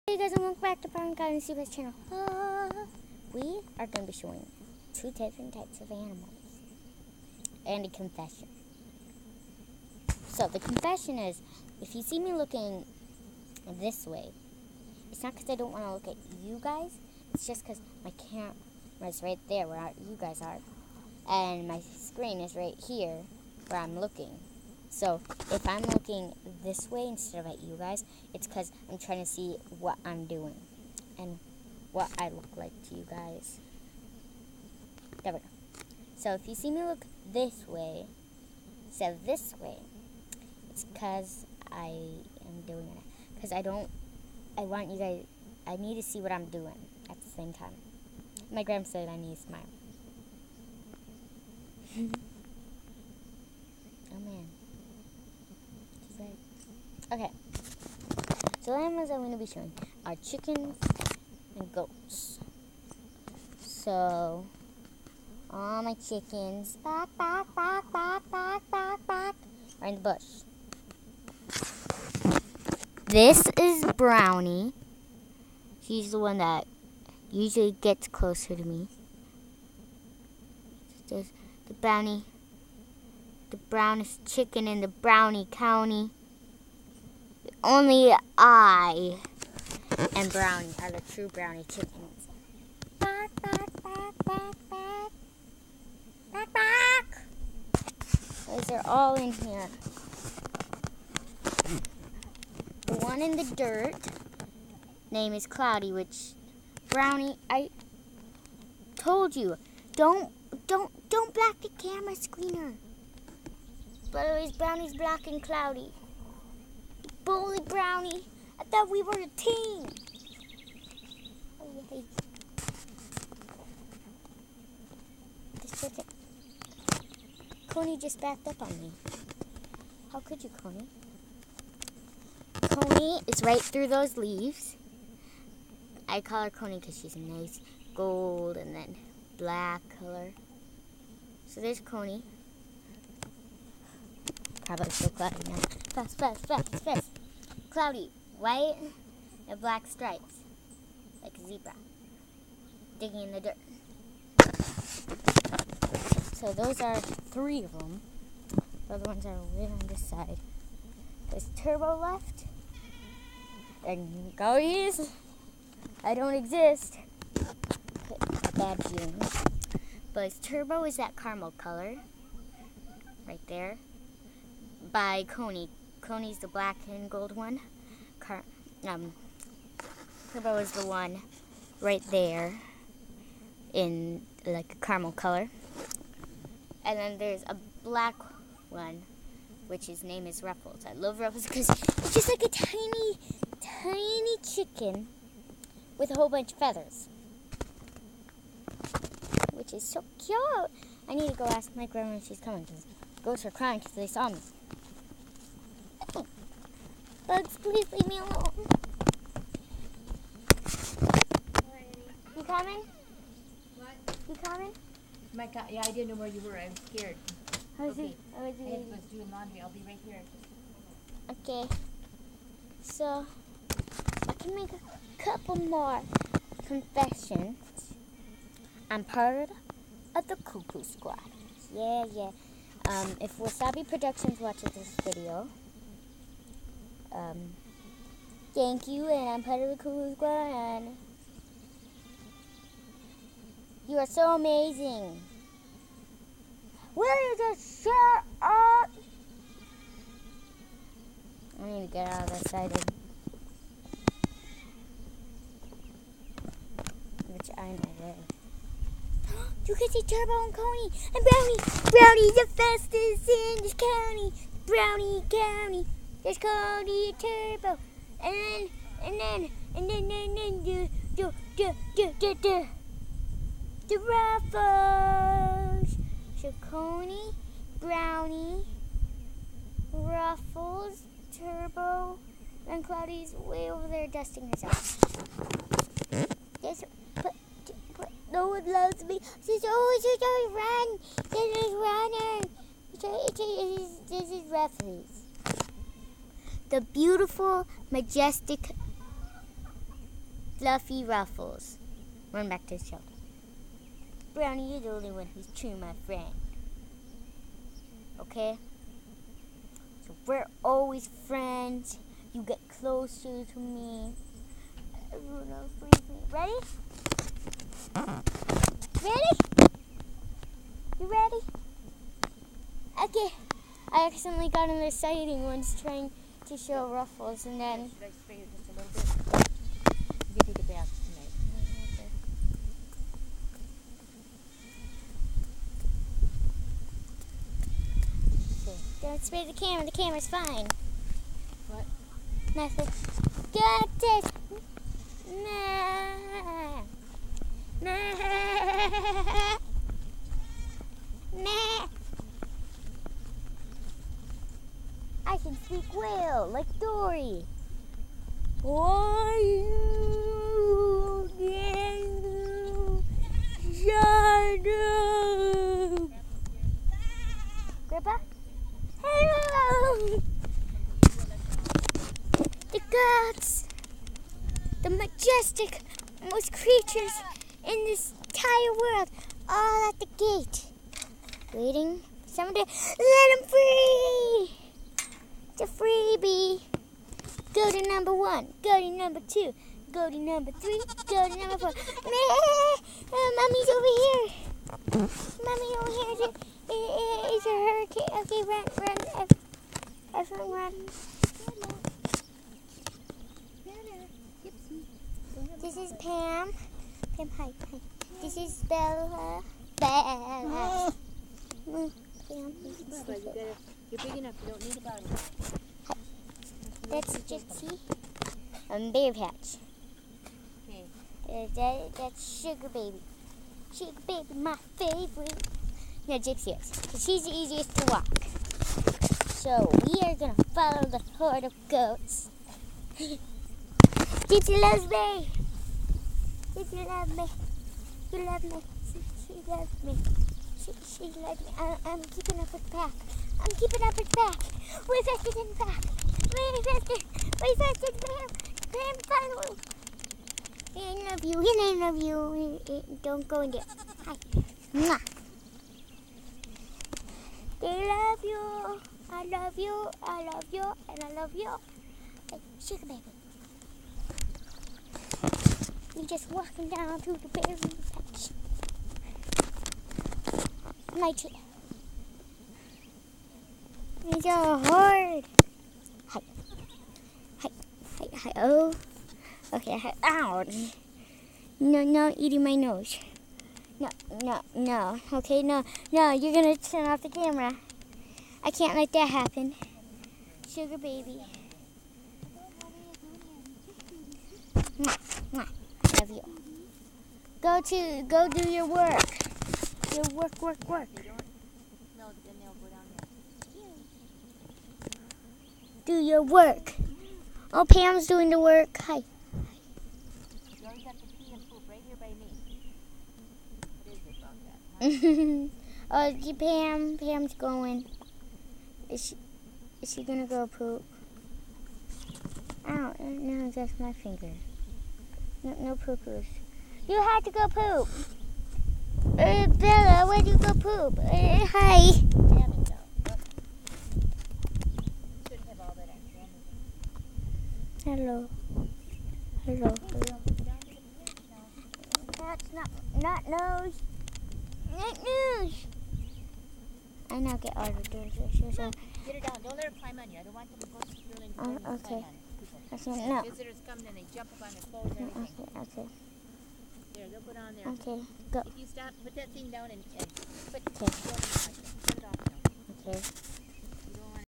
Chickens and goats
Goats names: Pam,Gypsie,Bella,Sugarbaby.
Chickens names: Ruffles,Cloudy,Brownie,Cony,Turbo.